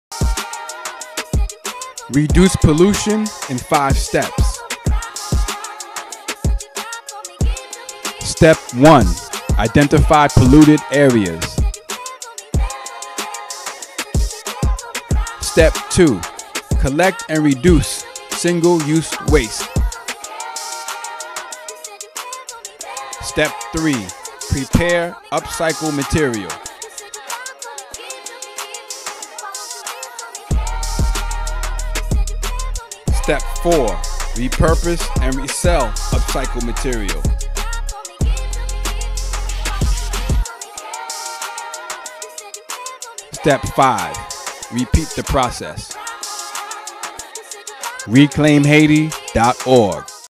RECLAIM HAITI - REDUCE PLASTIC POLLUTION IN 5 STEPS
REDUCING PLASTIC POLLUTION IN HAIIT IS NOT EASY. HOWEVER, IT"S NOT IMPOSSIBLE EITHER...

I'VE PERSONALLY WENT TO HAITI & RESEARCHED (for over 4 years) THE MOST BASIC STEPS TO REDUCE PLASTIC WASTE.. NOW I ASK ANYONE WITH LOVE FOR HAITI, TO RUN WITH THE "RECLAIM HAITI" PLAN AND DO YOUR PART!

 RECLAIM HAIIT - IT"S MORE THAN JUST RECYCLING!